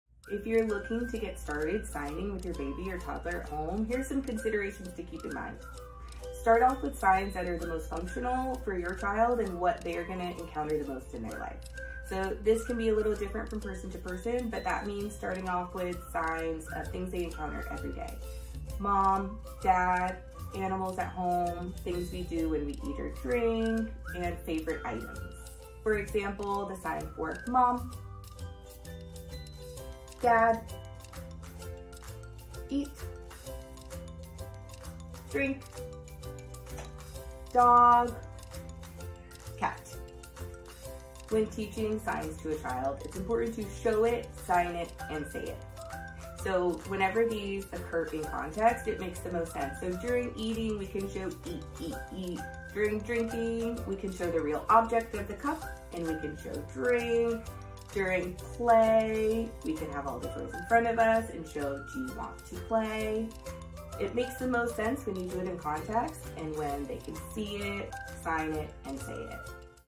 Baby Sign Language | Stephanie Farrow
Show it, sign it, say it. Stephanie Farrow, a speech-language pathologist with Orlando Health Arnold Palmer Hospital for Children Pediatric Rehabilitation Program, shares tips for getting started with baby sign language.